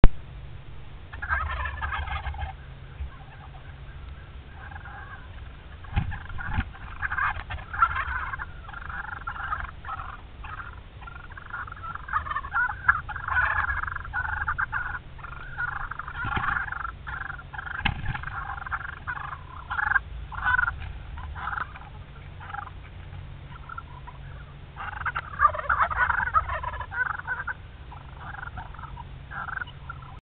Turkeys Gobbling.AVI
A great video of turkeys gobbling from our new M-80 Black courtesy of our friend, Willie, at OutdoorFreaks.net!